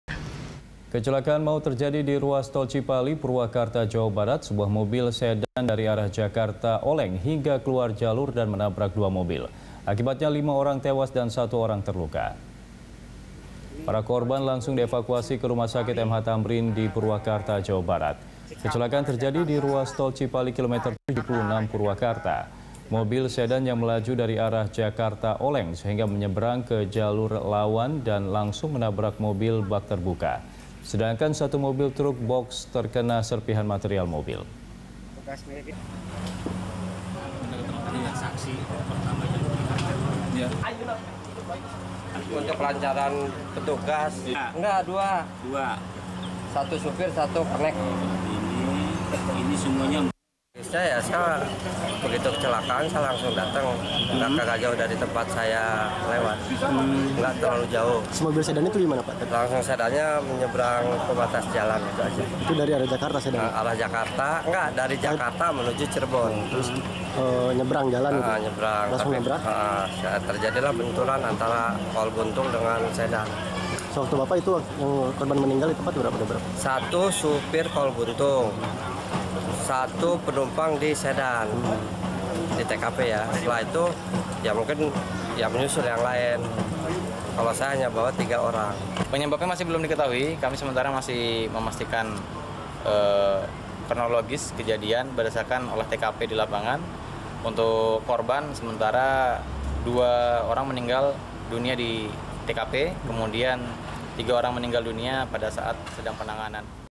Lima Orang Tewas dalam Kecelakaan di Tol Cipali
Lima orang tewas dalam kecelakaan maut di ruas Tol Cipali, Purwakarta, Jawa Barat. Sebuah mobil sedan dari arah Jakarta oleng hingga keluar jalur dan menabrak dua mobil.

Facebook.com/BeritaSatuTV
Youtube.com/BeritaSatu